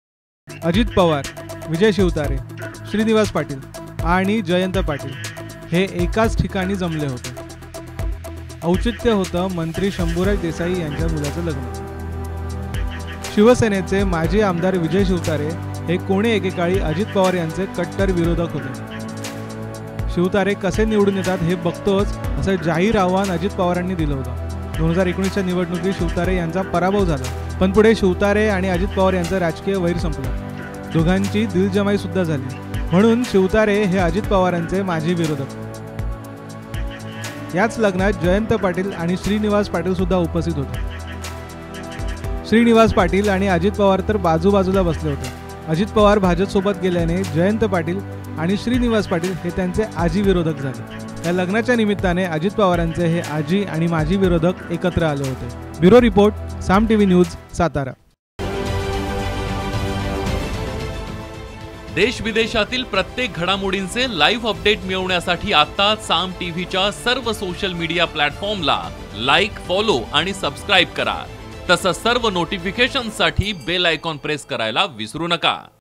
Ajit Pawar & Opposition on Same Function | जयंत पाटील आणि अजित पवार एकाच मंचावर
Ajit Pawar & Opposition on Same Function | अजित पवार भाजपसोबत गेल्याने जयंत पाटील आणि श्रीनिवास पाटील हे त्यांचे आजी विरोधक झालेत. शंभुराज देसाई यांच्या मुलाच्या लग्नाच्या निमित्ताने अजित पवारांचे हे आजी आणि माजी विरोधक एकत्र आले.

SRK